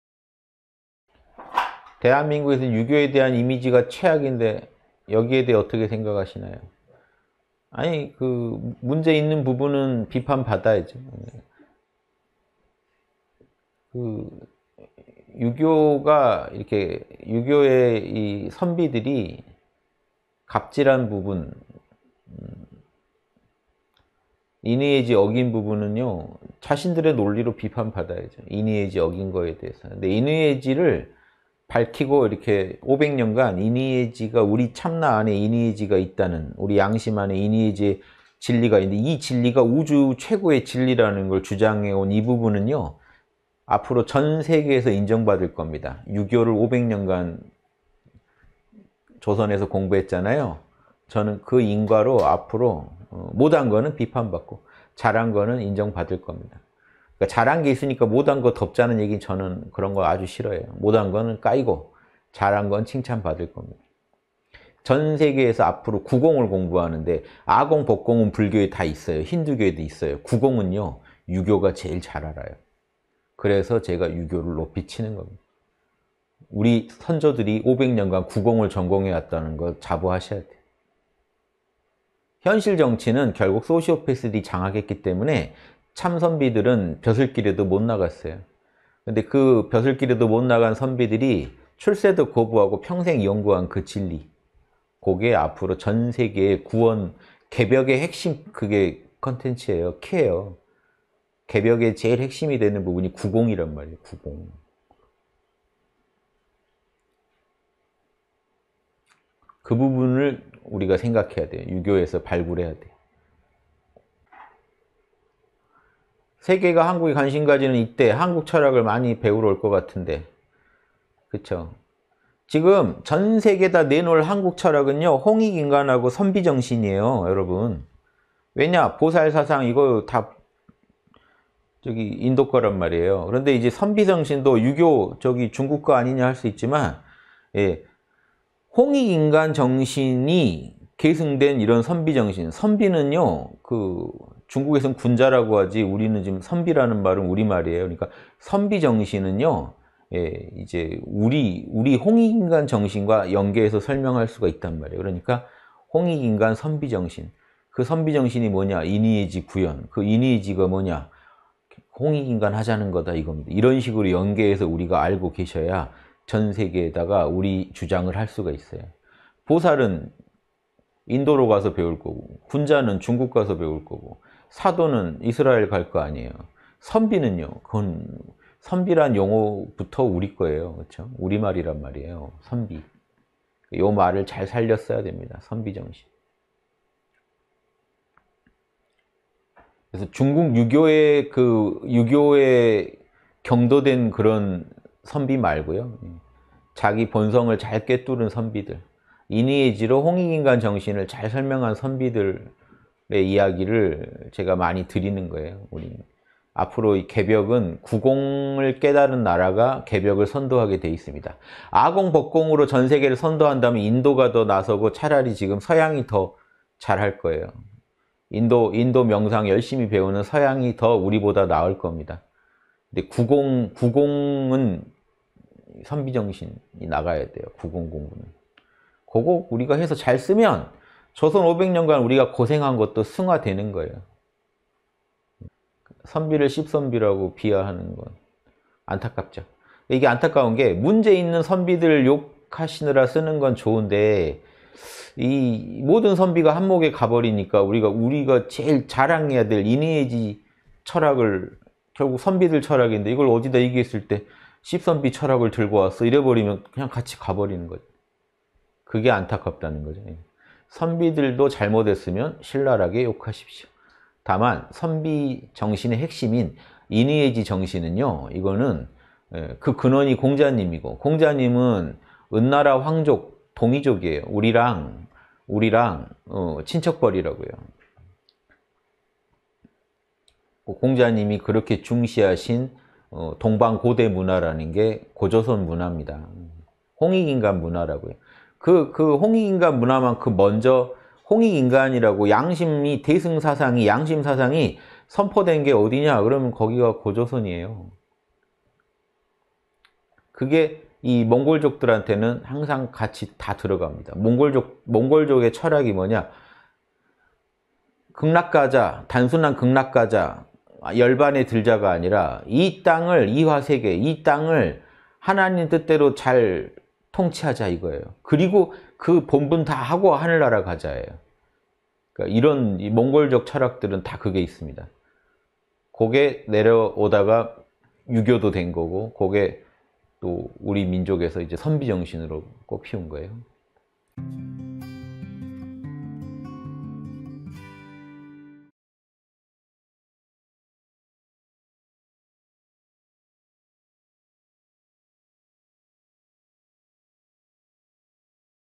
[양심 인문학] 선비 정신으로 개벽을 선도하라!
(강의 中) 유교의 선비들이 갑질한 부분 인의예지 어긴 부분은 자신들의 논리로 비판받아야죠. 그런데 인의예지를 500년간 밝히고 참나 안에 인의예지가 있고 이것이 우주 최고의 진리라는 것을 주장해 온 이 부분은요. 앞으로 전 세계에서 인정받을 겁니다.